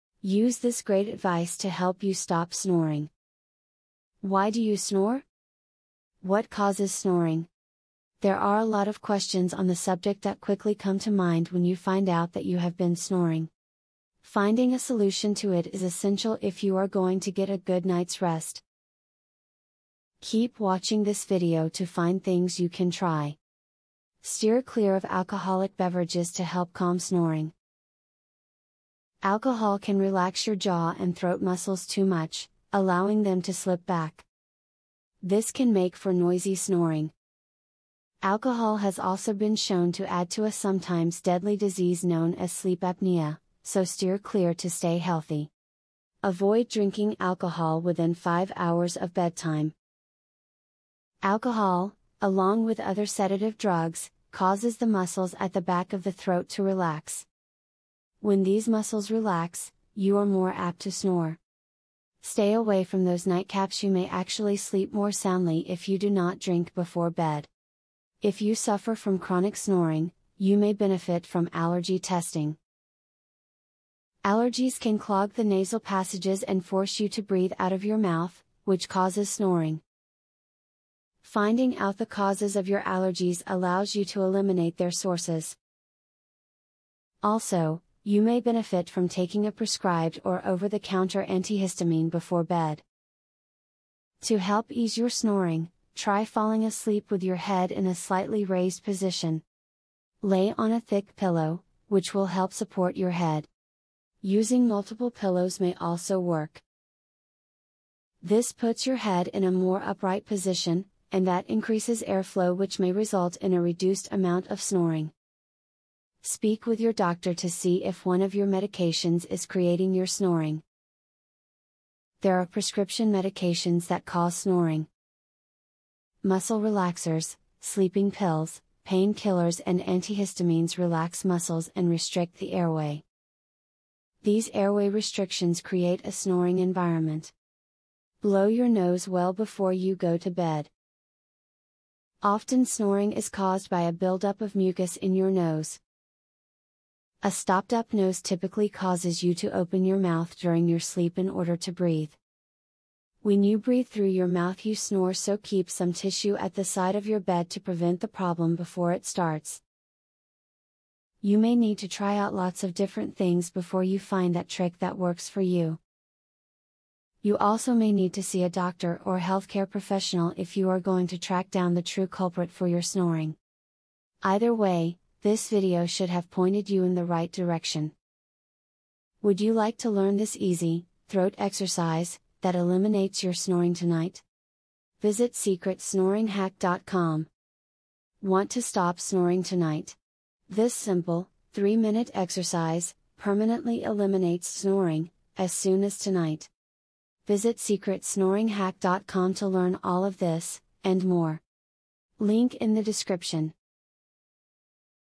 Use This Great Advice To Help You Stop Snoring.
Use This Great Advice To Help You Stop Snoring. Why do you snore? What causes snoring? There are a lot of questions on the subject that quickly come to mind when you find out that you have been snoring. Finding a solution to it is essential if you are going to get a good nights rest.